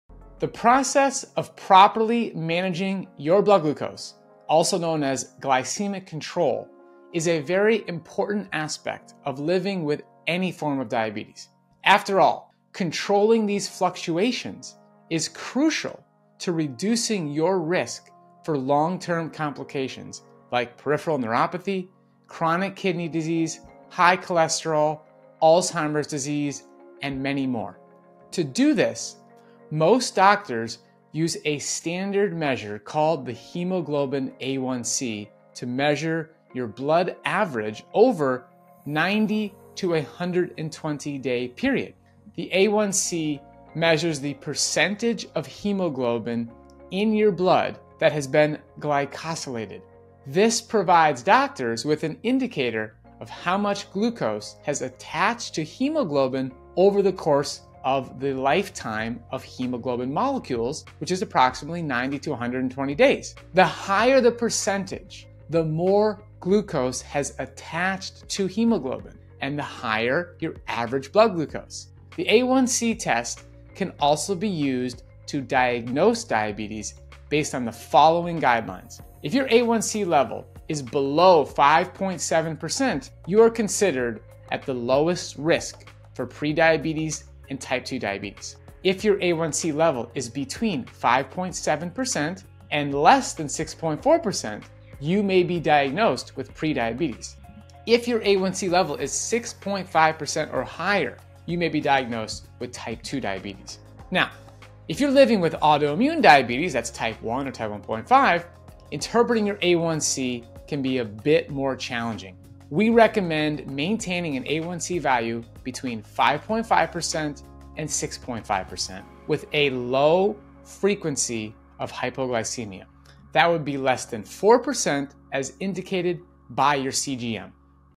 MUST-KNOW A1C Guidelines for Blood Glucose Management | Mastering Diabetes
Improve your glycemic control and reduce diabetes-related risks with our comprehensive guide! Discover the significance of the A1C test in measuring your blood glucose levels over time. Learn how to interpret A1C results and find out your diabetes risk.

If you have type 1 or type 1.5 diabetes, we'll provide tailored advice for optimal A1C values.